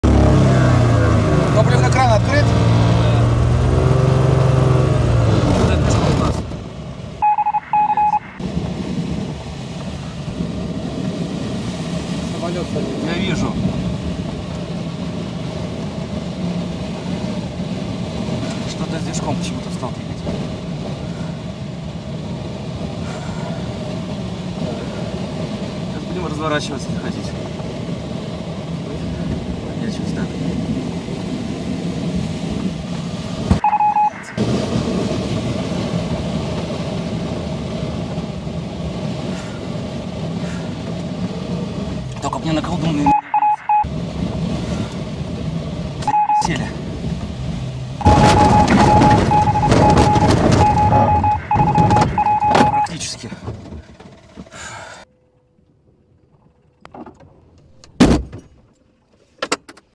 Аварийная посадка планера DG 500 M
Мотор мертв, Вепрь цел! Сегодняшняя посадка стоила мне всех моих навыков. Отсмотрел ролик. Нашел секугнду, на которую можно было развернуться раньше и, возможно избежать циркуля. Но это не точно :)
Нас можно поддержать добрым словом и добрым делом :)
Всегда  признателен за разумные комментарии, отзывы, советы и конструктивную критику.Ваш небесный гид
Игорь Волков.